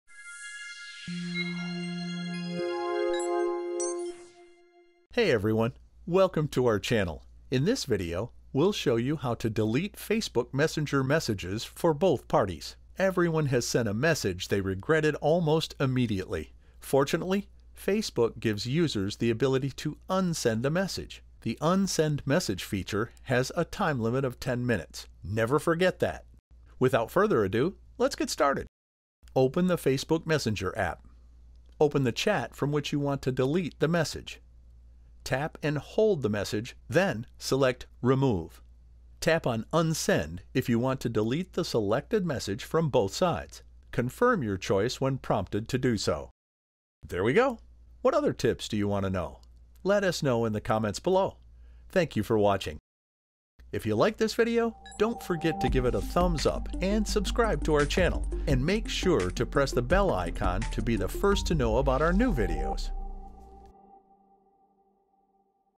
How to Delete Facebook Messenger Messages for Both Parties
In this video, we’ll show you How to Delete Facebook Messenger Messages for Both Parties.

Everyone sent a message they regretted almost immediately. Fortunately, Facebook gives users the ability to «unsend» a message. The unsend message feature has a time limit of 10 minutes. Never forget that.

Without further ado, let’s get started.

1)  Open the Facebook messenger app.
2) Open the chat from which you want to delete the message.
3) Tap and hold the message, then select Remove.
4) Tap on Unsend if you want to delete the selected message for both sides.
5) Confirm your choice when prompted to do so.